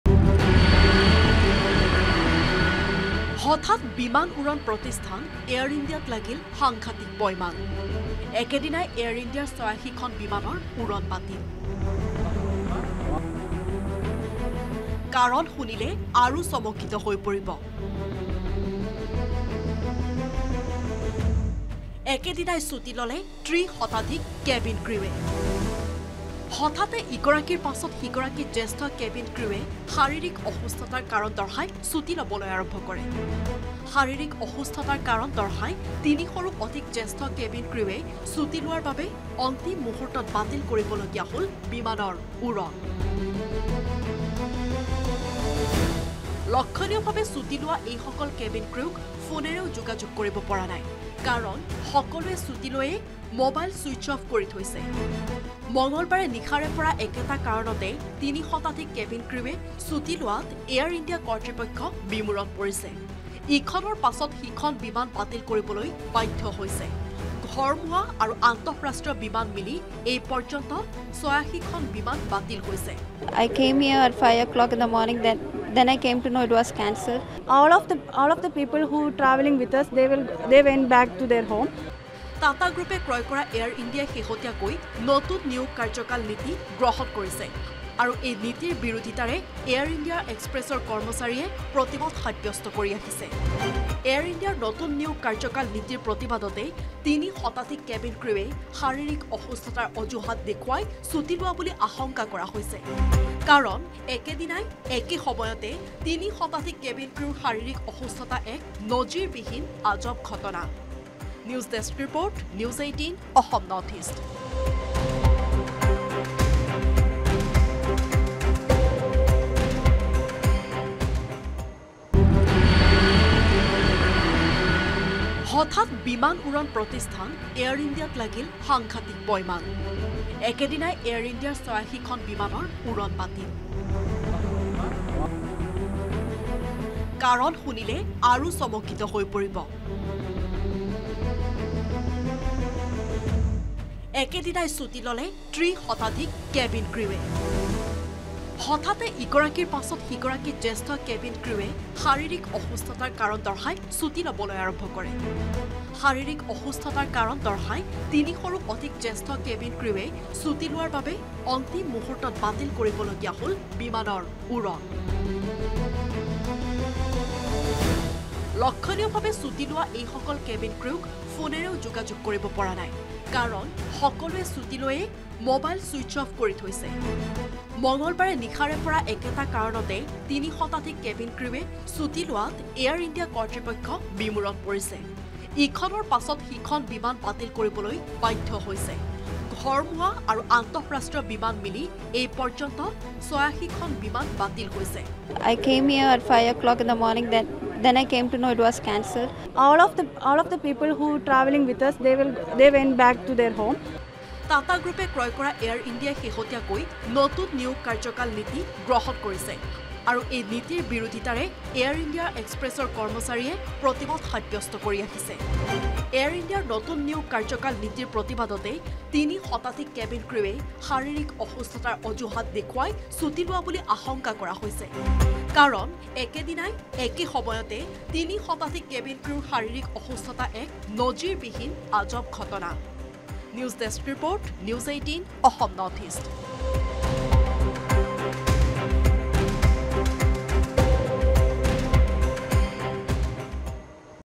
86 Air India Express flights cancelled | এয়াৰ ইণ্ডিয়াৰ ৮৬খন বিমানৰ উৰণ বাতিল N18V
86 Air India Express flights cancelled | এয়াৰ ইণ্ডিয়াৰ ৮৬খন বিমানৰ উৰণ বাতিল । এয়াৰ ইণ্ডিয়াৰ তিনিশৰো অধিক জ্যেষ্ঠ কেবিন ক্ৰুয়ে অসুস্থতাৰ কাৰণ দৰ্শাই ছুটি লোৱাৰ বাবে অন্তিম মুহূৰ্তত বাতিল কৰিব লগীয়া হ’ল বিমানৰ উৰণ। হঠাতে ইগৰাকীৰ পাছত সিগৰাকী জ্যেষ্ঠ কেবিন ক্ৰয়ে শাৰিৰীৰ অসুস্থতাৰ কাৰণ দৰ্শাই ল’লে ছুটি ।